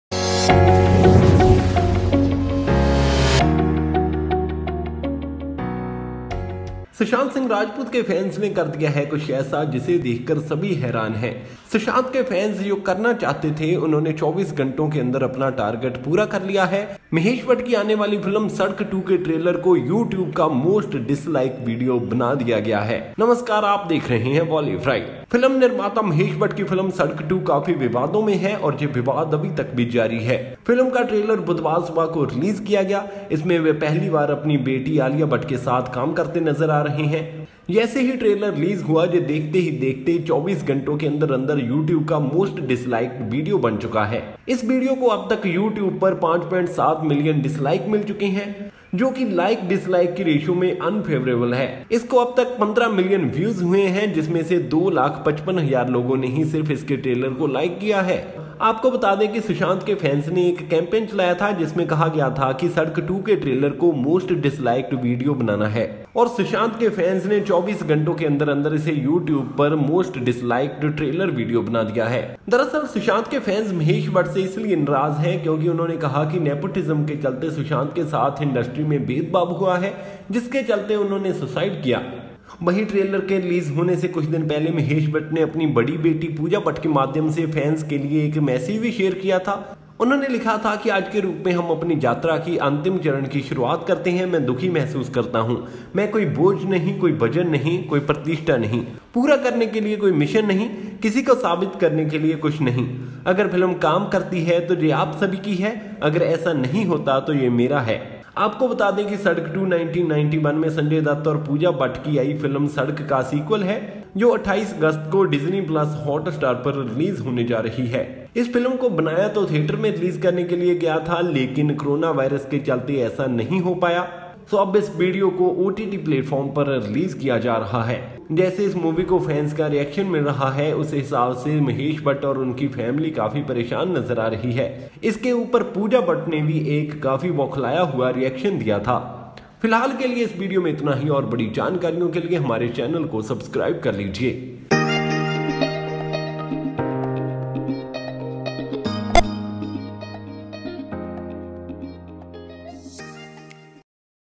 Sadak 2 the most disliked trailer on YouTube amid nepotism debate | Bolly Fry
Sadak 2 become MOST DISLIKED Trailer Video On Youtube. Mahesh Bhatt and Alia Bhatt’s Sadak 2 trailer has garnered the dubious distinction of becoming the most disliked trailer on YouTube.